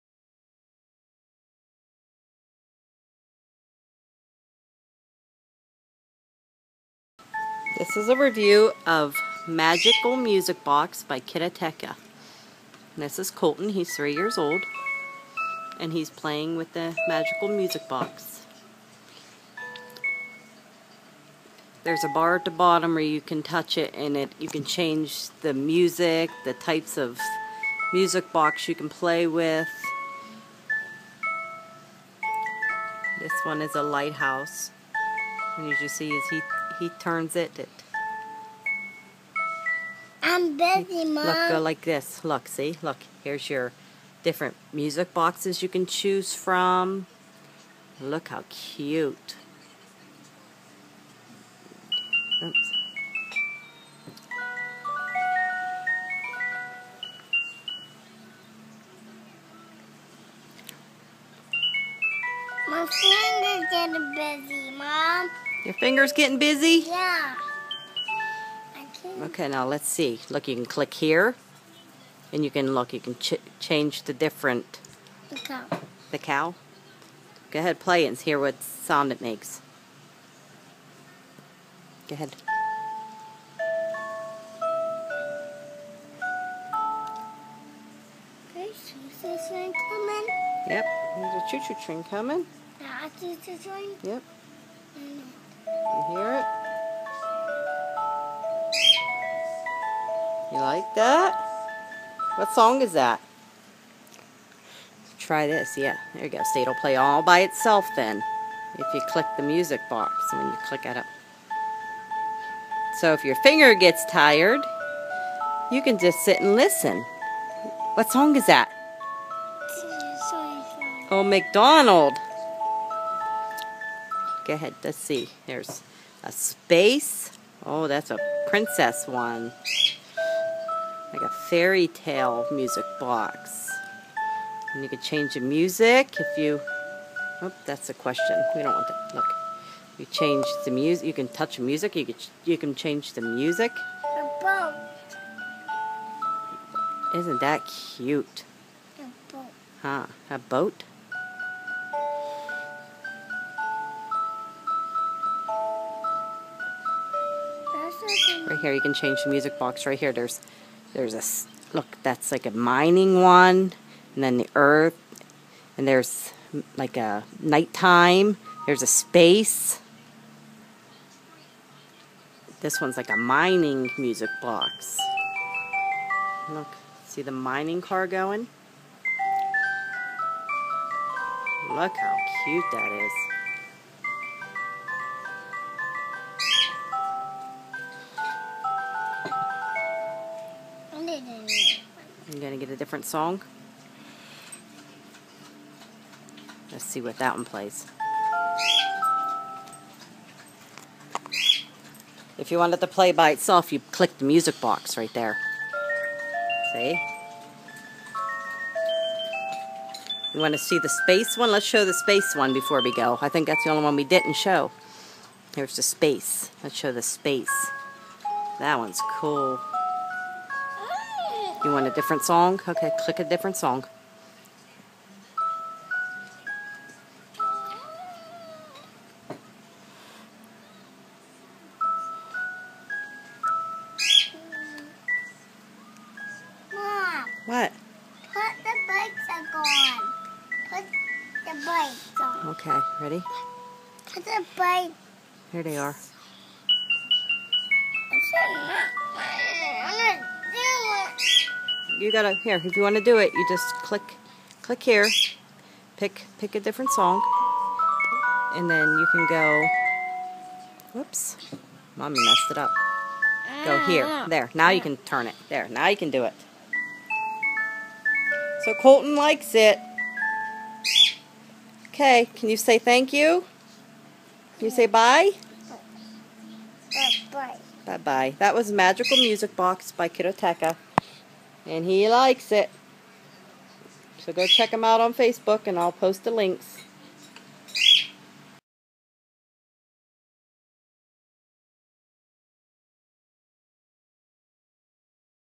Magical Music Box Review
Today's What in the World are they playing is Magical Music Box by Kidoteca

From the opening screen Magical Music Box the animations are spectacular and graphics
are crisp and crystal clear, it's like stepping into another land.

There are 6 different Music Boxes to choose from:
A train in the countryside
A ship traveling on the ocean
A city at night
Gnomes in the diamond mine
A beautiful carousel scene
A solar system.

There are also a bunch of different songs to choose from each time you pull up the Menu that is located on the bottom you have a variety of different songs that are categorized by type.
This app can be played on manual or automatic and if you click the information button it will give you the lyrics to song.

Overall this is a beautifully illustrated and animated app gives you lots of options to change the music box type and music, learn the lyrics. I highly recommend Magical music box